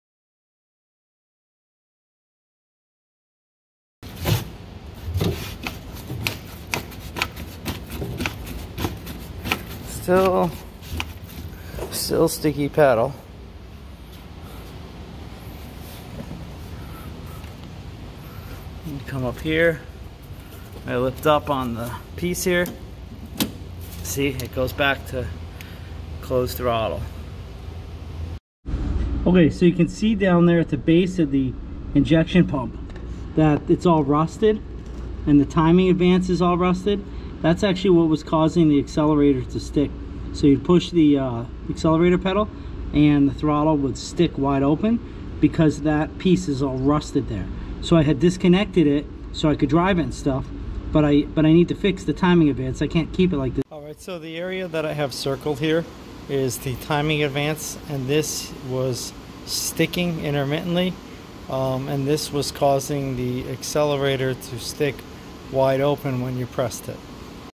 Unintended Acceleration GM 6.5 Diesel HMMWV military Humvee with a DB2 Injection pump. Hummer H1
This problem could happen to the 6.2 and GM 6.5 Diesel used in the hummer H1, GM Silverado 2500 , 3500 and 4500, Ford F250 Ford F350 Ford F450, GMC 2500 , 3500 and 4500 Topkick Dodge ram 2500 and 3500 with the Cummins, The subject vehicle was a Military M1114 Humvee decommissioned from the marine corps. I found the problem before I ever started the engine. This could happen to any vehicle with a stanine injection system.